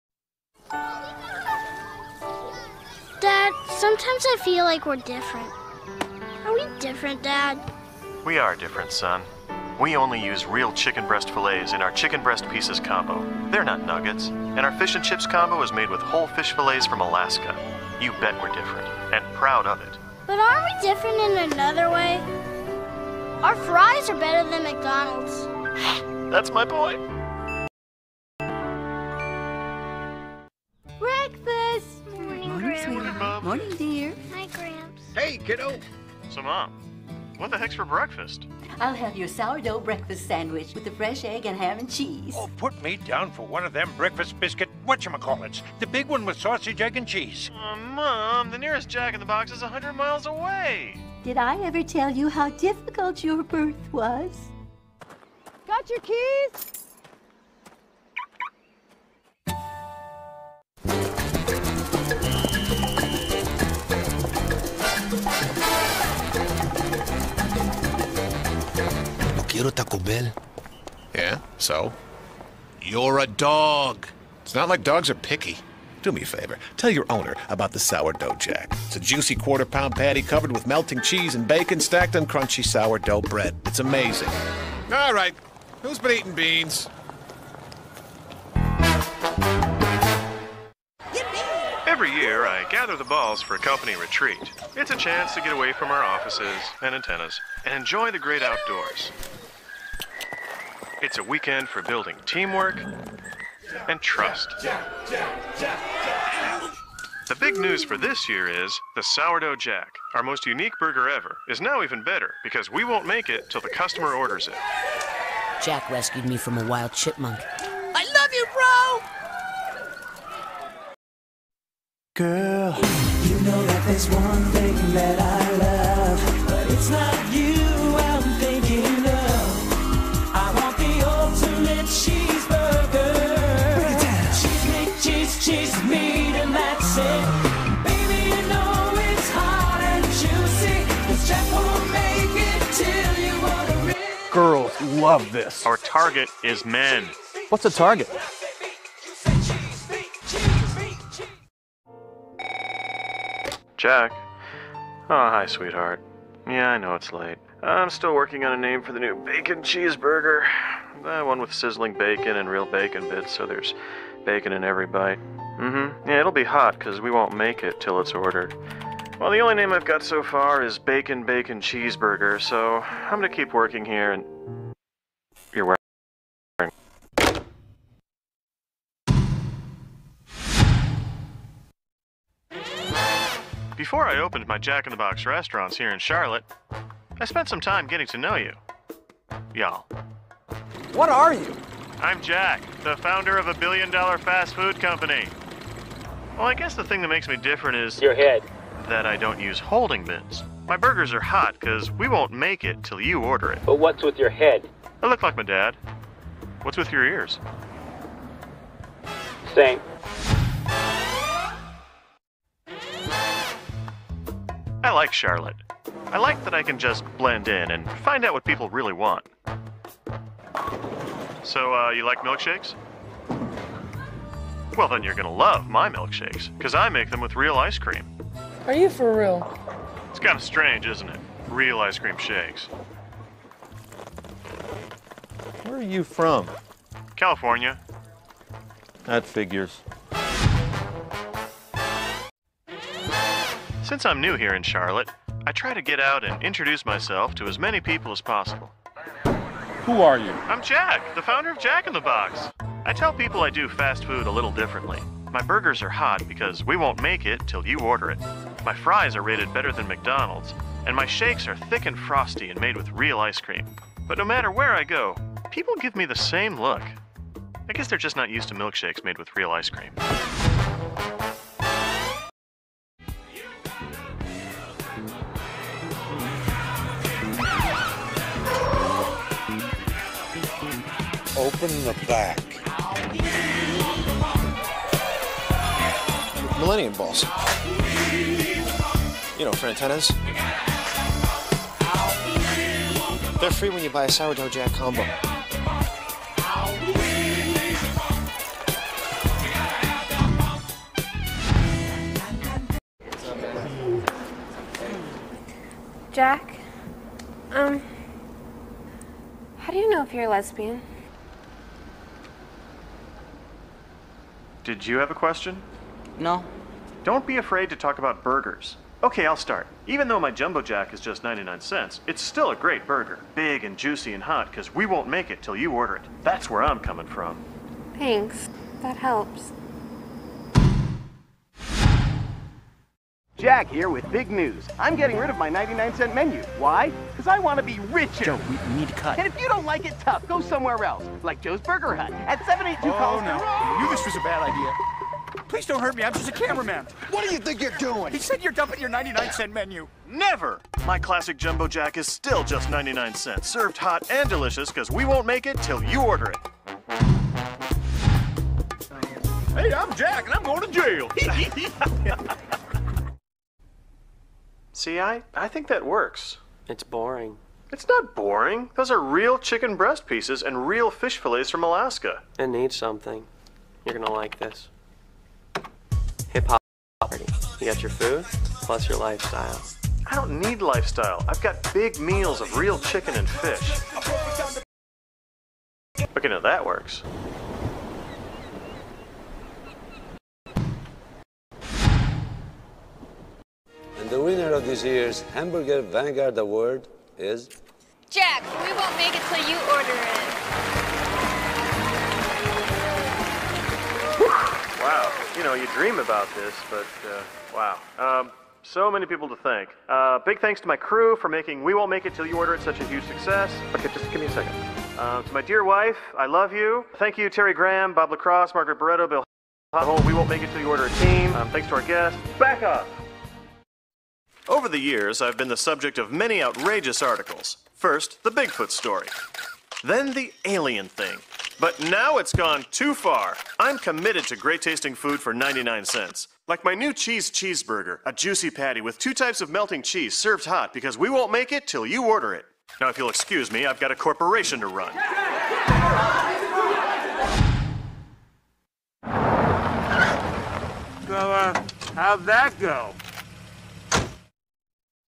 The Jack In The Box Show - Season 5 Commercial Compilation (1999-2000)
0:00 S5 E1: That's My Boy
0:30 S5 E2: Breakfast
1:01 S5 E3: Taco Bell Dog
1:31 S5 E4: The Great Outdoors
2:01 S5 E5: Melty Cheese Love
2:32 S5 E6: Late Night Work
3:02 S5 E7: Visiting Charlotte Pt. 1
3:32 S5 E8: Visiting Charlotte Pt. 2
4:02 S5 E9: Visiting Charlotte Pt. 3
4:33 S5 E10: Antenna Ball Dance Party
5:03 S5 E11: Lesbian
5:33 S5 E12: Impersonating (also seen in Season 6 Episode 1)
6:03 S5 E13: Background
6:33 S5 E14: Award Ceremony
7:04 S5 E15: Articles